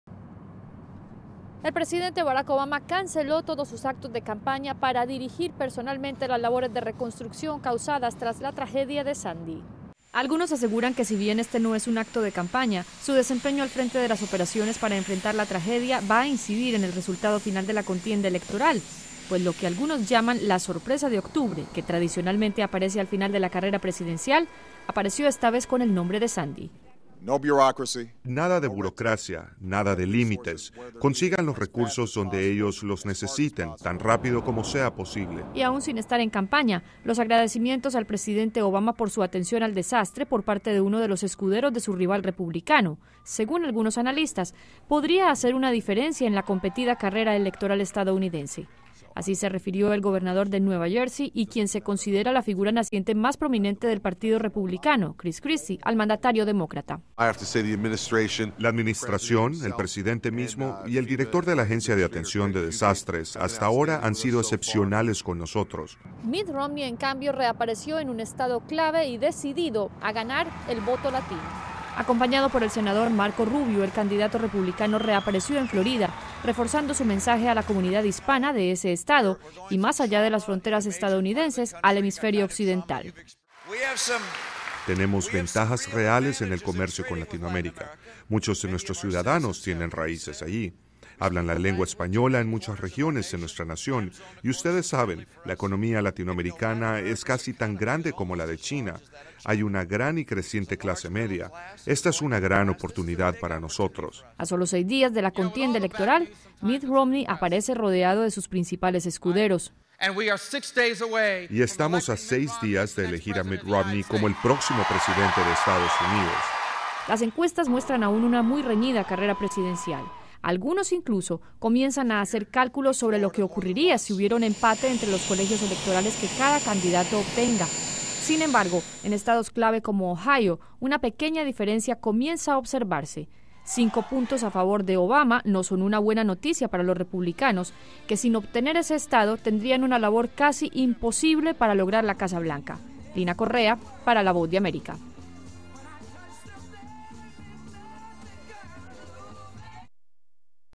Republicanos retoman campaña en Florida
Mientras el presidente Barack Obama suspendió todo acto de campaña para dedicarse a atender la emergencia generada por Sandy, el candidato republicano, Mitt Romney, retomó su carrera presidencial en Florida.  Allí, al lado de Marco Rubio, apareció para impulsar el voto latino en uno de los estados cruciales de la elección presidencial que se realizará en 6 días.